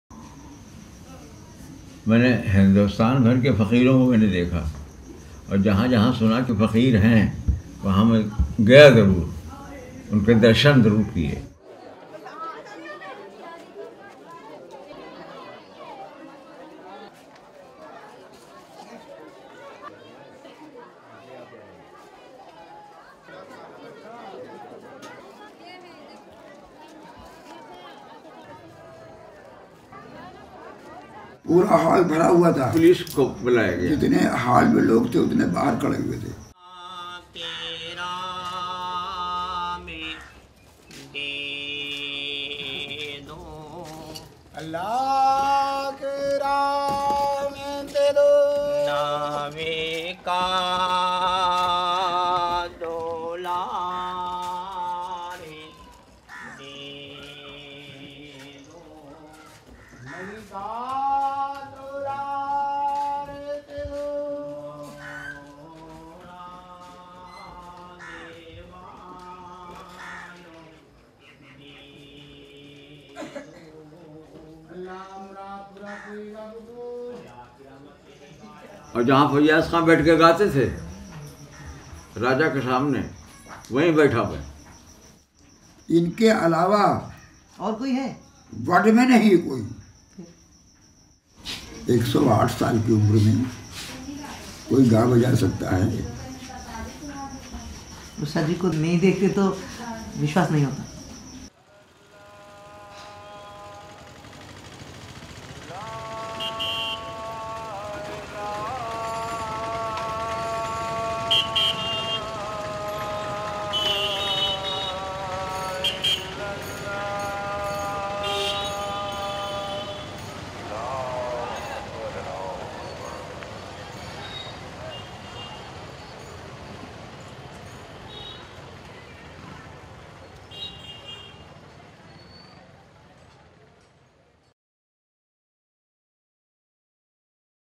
Abheri De ~ cinematography showreel # Docu USTAAD ABDUL RASHID KHAN
Ustaad Abdul Rashid Khan, the legendary Hindustani classical vocalist who died at the age 109, in 2016. this is the glimpse of an unfinished work.